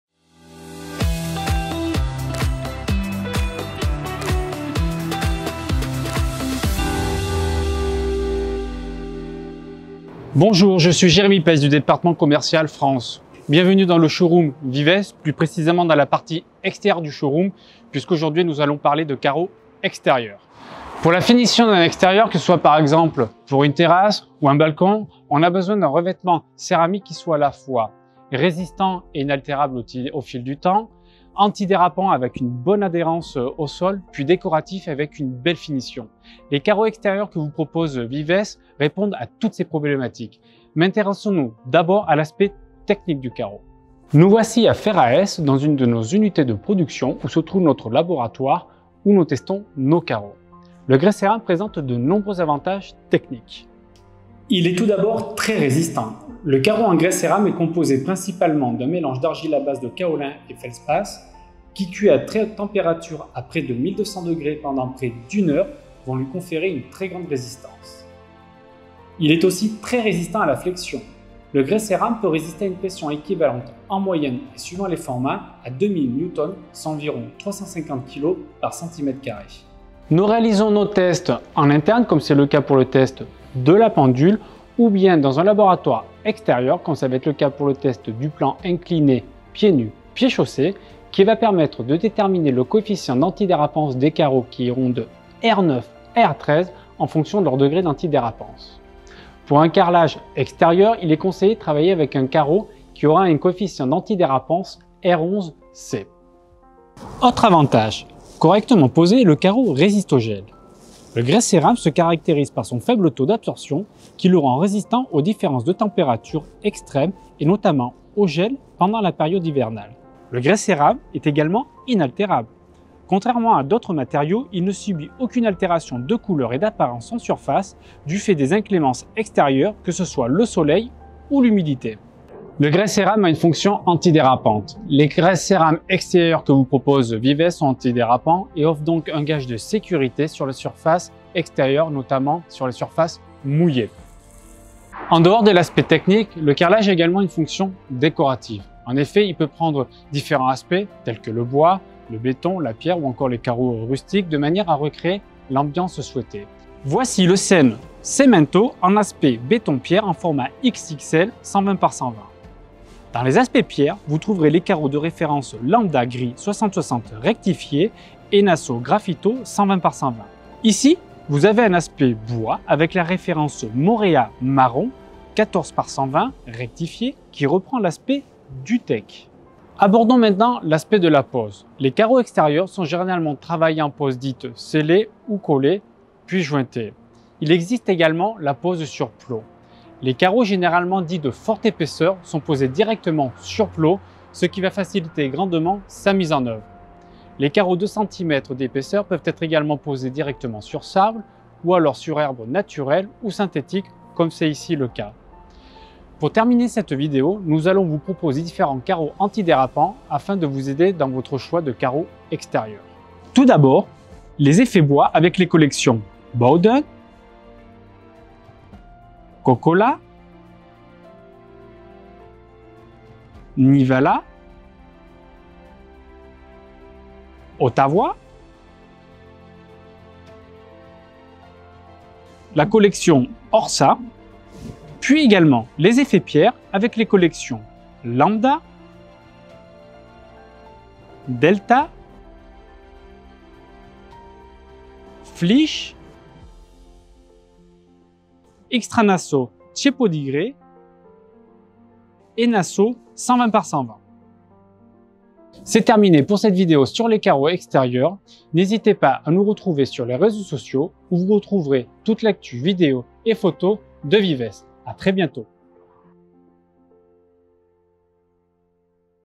VIVES. Solutions céramiques pour espaces extérieurs.
Parce que le carrelage est un matériau polyvalent avec des applications infinies, et l'un des plus pratiques et des plus précieux est celui des espaces extérieurs. Jérémie Peis, notre responsable de zone en France, explique ses avantages, les différentes propositions et les normes de qualité auxquelles sont soumis les carreaux d'extérieur de Vives.
Because ceramic is a versatile material with endless applications, and one of the most practical and most valuable is in outdoorareas. Jérémie Peis, our areamanager in France, explains its advantages, the different proposals and the quality standards to which Vives ceramic materials are subjected.